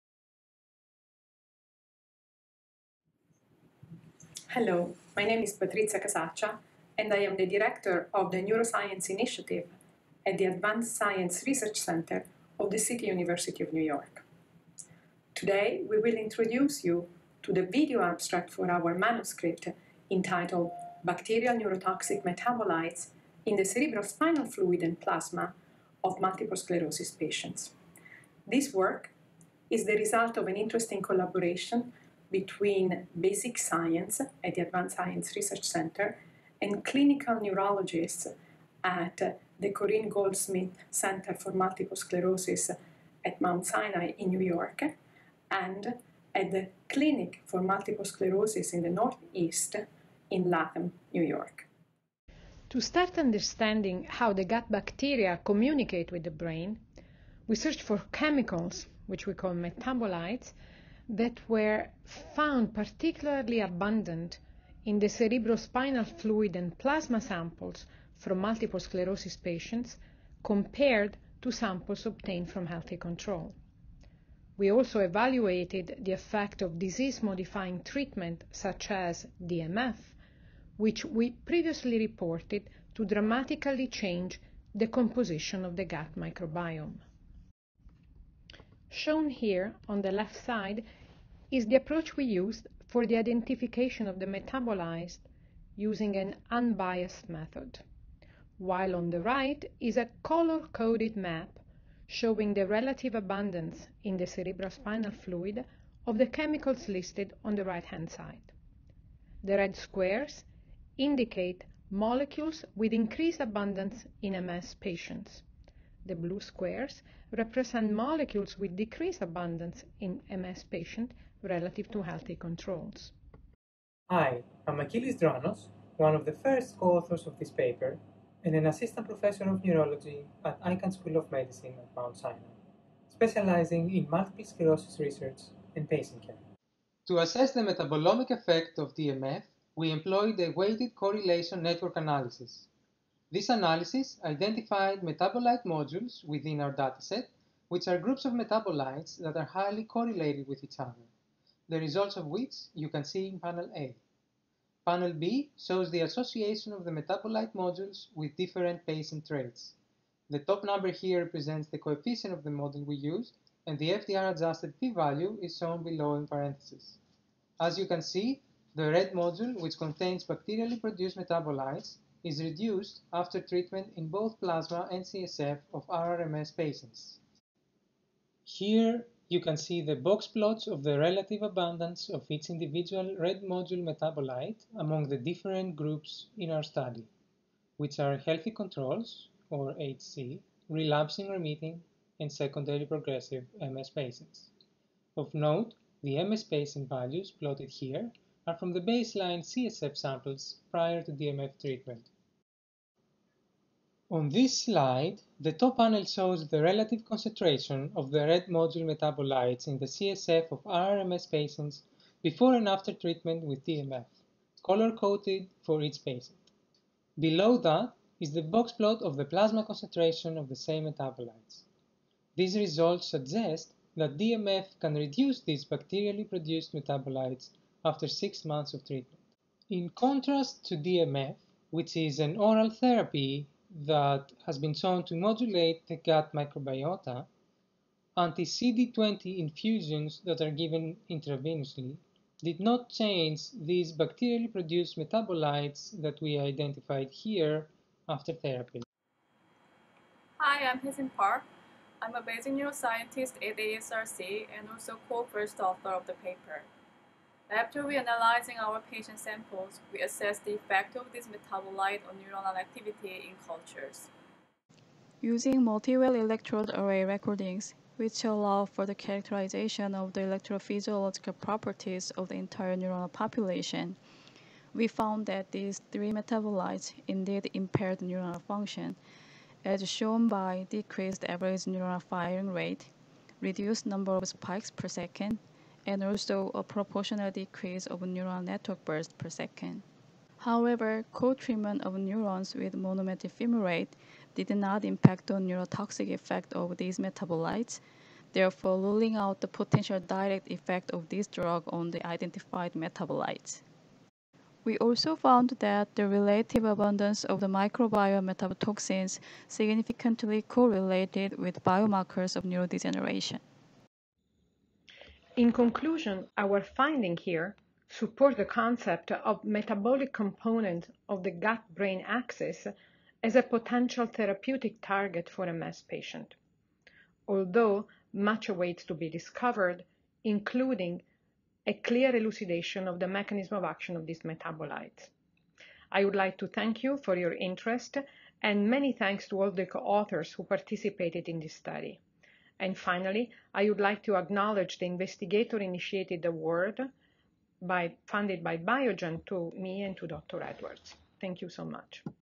Bacterial neurotoxic metabolites in multiple sclerosis cerebrospinal fluid and plasma
Ntranos et al. perform metabolomic analysis of CSF and plasma samples from multiple sclerosis patients. They identify phenol and indole derivatives from the catabolism of tryptophan and phenylalanine as microbially derived, potentially neurotoxic metabolites that may mediate gut-brain communication in multiple sclerosis.